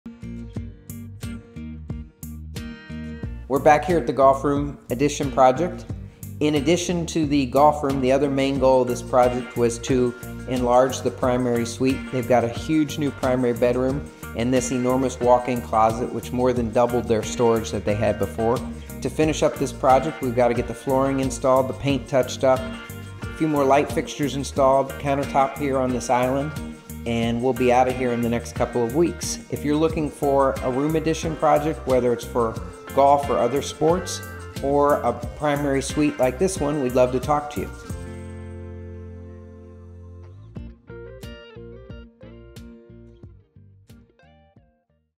August Update - Custom Luxury Home Remodel: Basement Golf Simulator and New Master Closet
As custom luxury home remodelers, we love when we have the opportunity to do something unique for our clients!